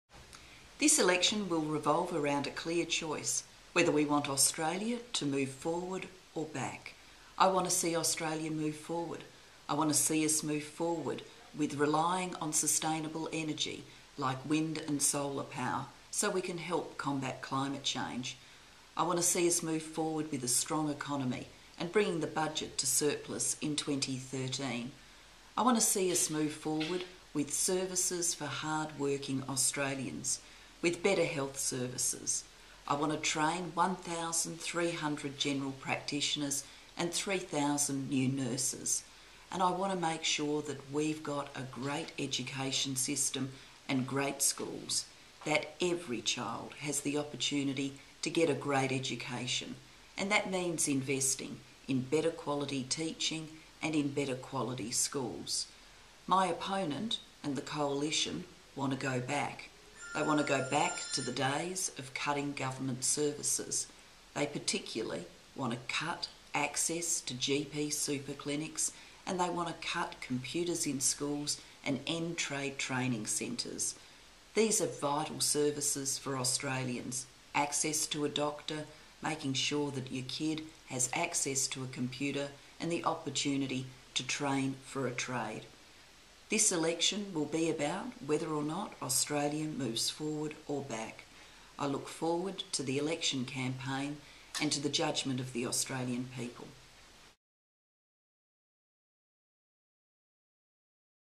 Together, let's move Australia forward
Today, I fulfil the pledge I made on the day I became Prime Minister. On that day, I acknowledged it is the right of every Australian to vote for the Prime Minister, and that in the very near future I would ensure all Australians could exercise their right.

I now seek my own mandate to move Australia forward.

I've just met with the Governor General, Quentin Bryce and asked her to call an election for August 21.

The Australian people will elect the Prime Minister of their choice on that day.

This election will revolve around a clear choice: whether we want Australia to move forwards or backwards.

I believe we must move forward by building a sustainable Australia, an Australia with a stronger economy, and an Australia with first rate health and education services.